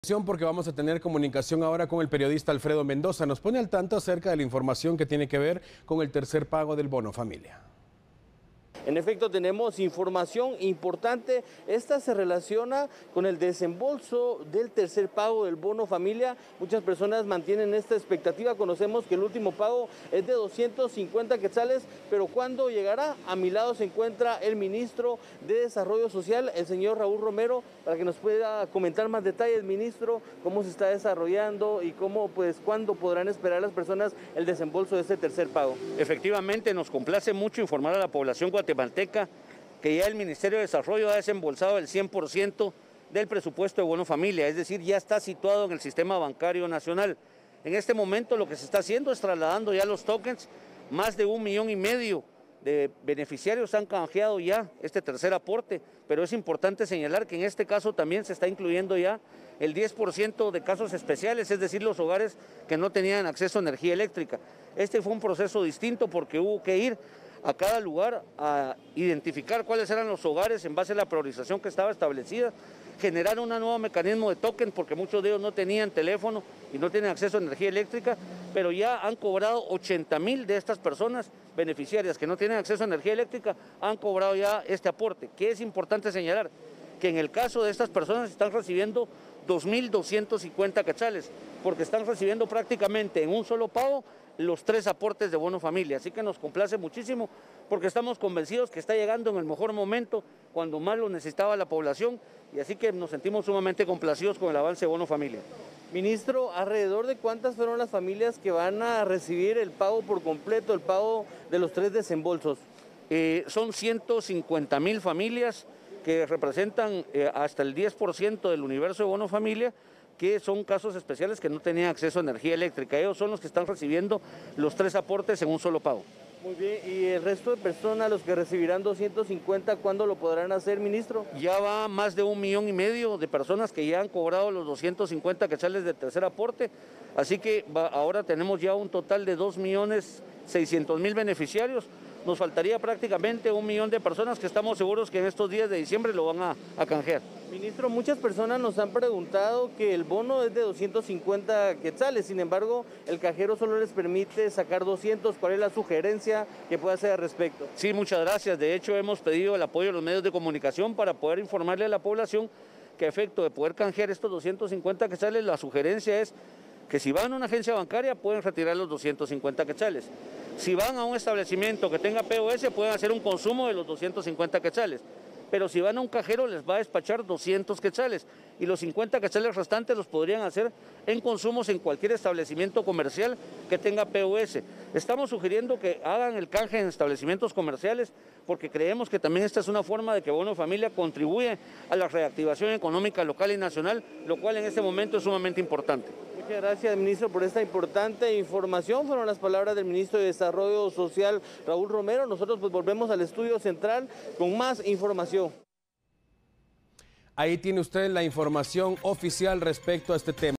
Conozca los detalles del tercer desembolso del Bono Familia
Más de millón y medio de beneficiarios ha recibido el aporte

 Nuestro compañero Alfredo Mendoza conversó con el Ministro de Desarrollo Social sobre el desembolso del tercer pago del bono familia.